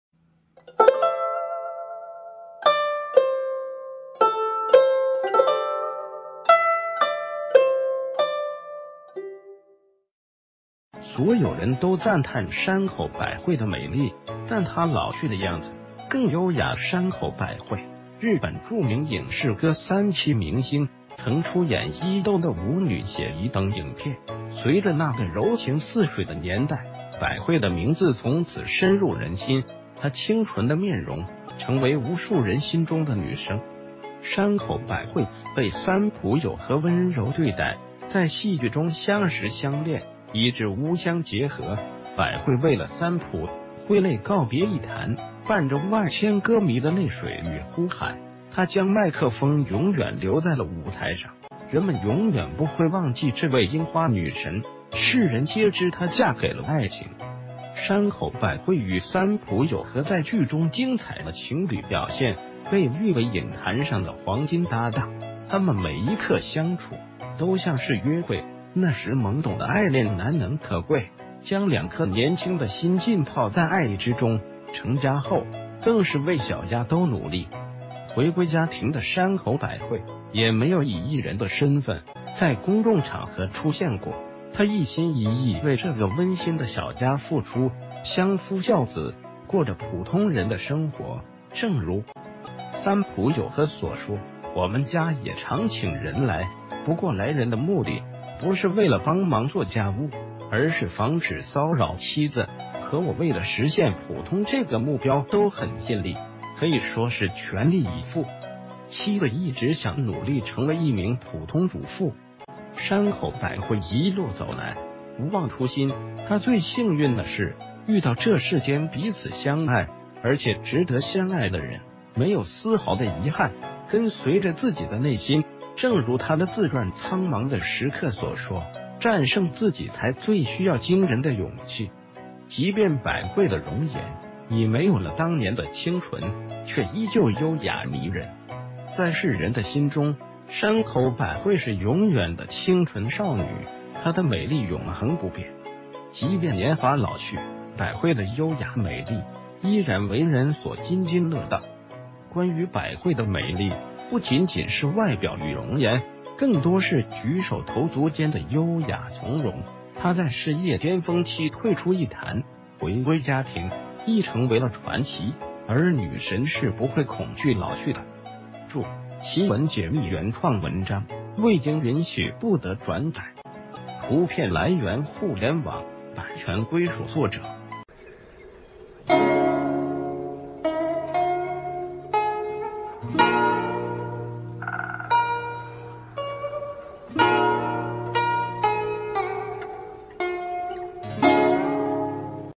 所有人都贊嘆山口百惠的美麗，但她老去的樣子更優雅
所有人都贊嘆山口百惠...